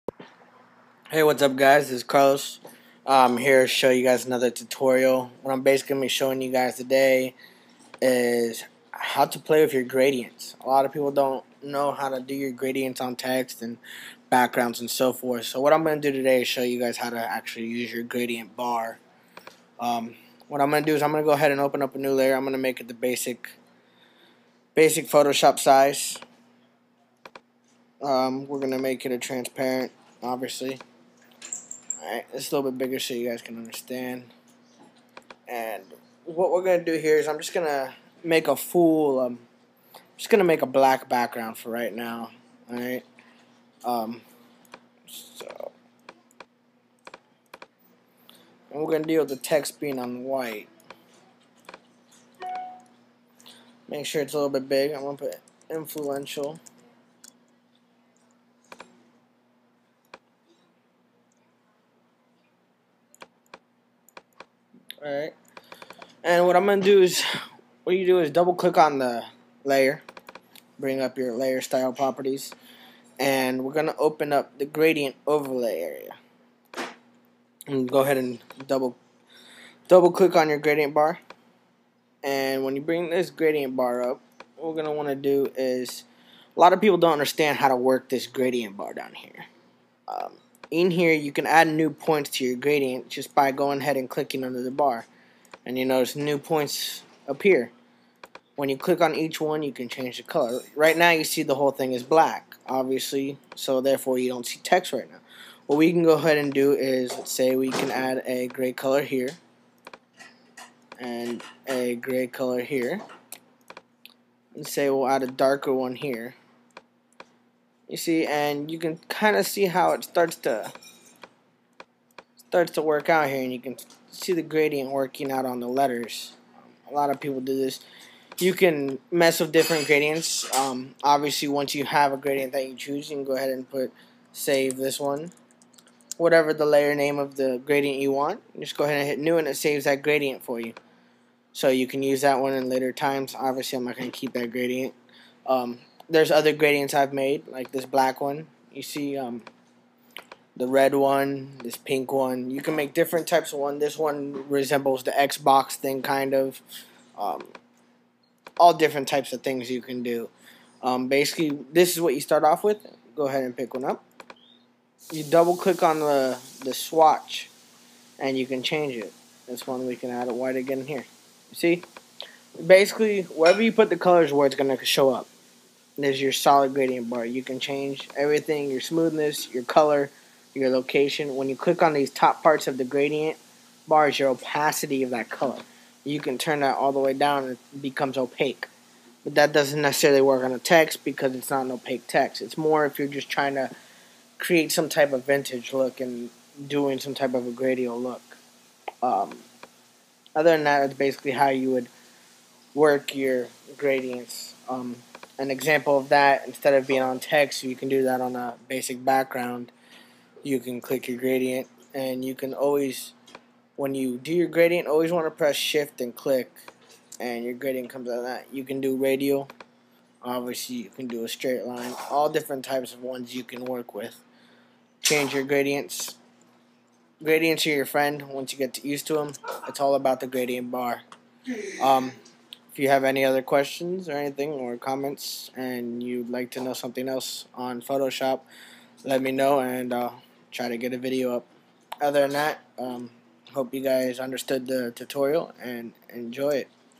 How to use Gradients in Photoshop
This tutorial is to show users how to use there gradient bars in text and on layers.